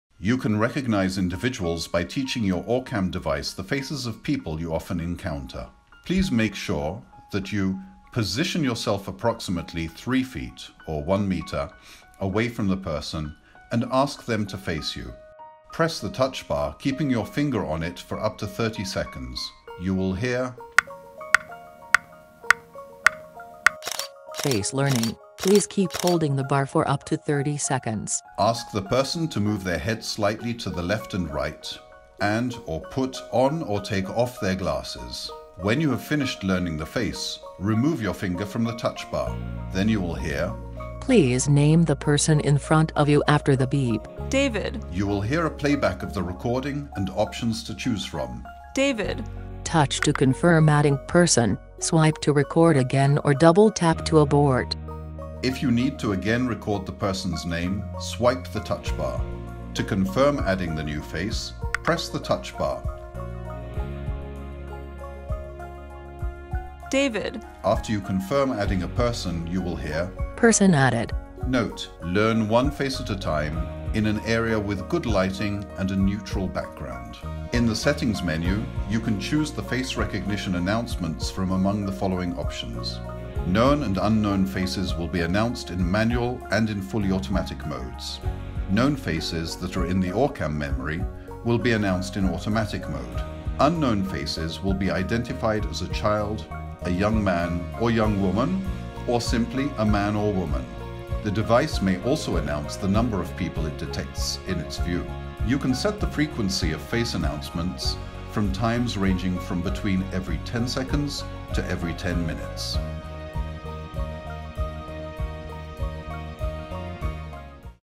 OrCam MyEye 2 Tutorial - Face Learning
How to recognize faces with OrCam MyEye 2.